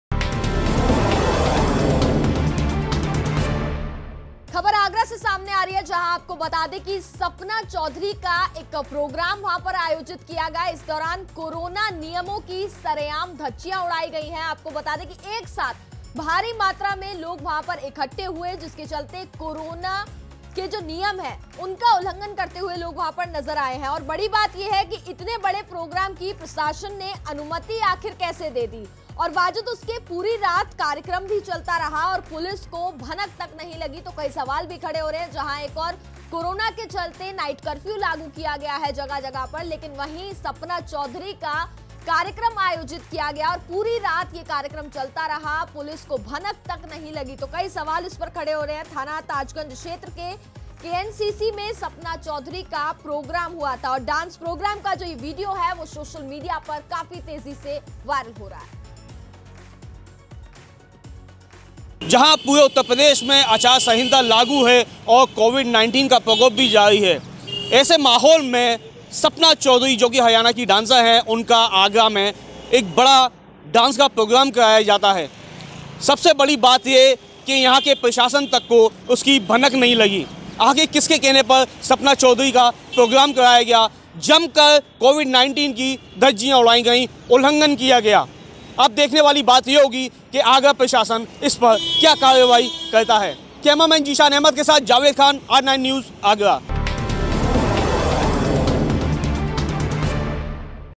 आगरा : सपना चौधरी के प्रोग्राम में कोविड-19 की उड़ाई गई धज्जियां | R9 News
शहर में सपना चौधरी के प्रोग्राम में कोविड-19 उड़ाई गई धज्जियां
प्रोग्राम के दौरान उमड़ी सैकड़ों लोगों की भीड़
पूरी रात चलता रहा प्रोग्राम पुलिस को नहीं लगी भनक
इतने बड़े प्रोग्राम की प्रशासन ने कैसे दी अनुमति
डांस प्रोग्राम का वीडियो सोशल मीडिया पर हुआ वायरल
थाना ताजगंज क्षेत्र के kncc में हुआ सपना चौधरी का प्रोग्राम